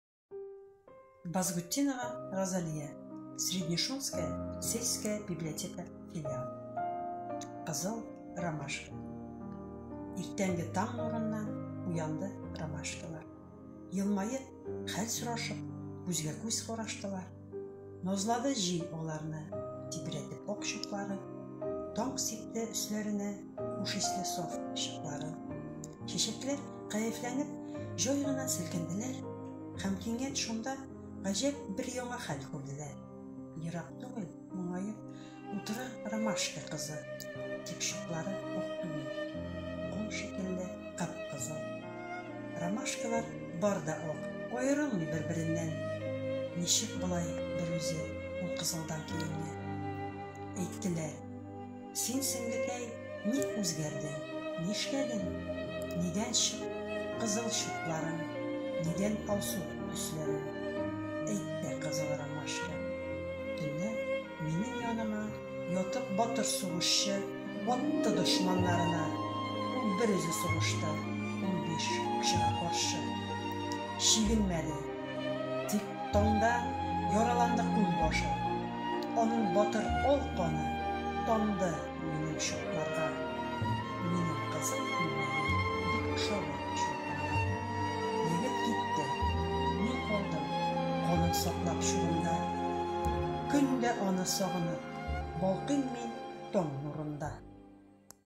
Базгутдинова Р.Х. "Кызыл ромашка". Среднешунская СБФ
Первый этап в Кировской области III Международного литературного конкурса чтецов «Джалиловские чтения»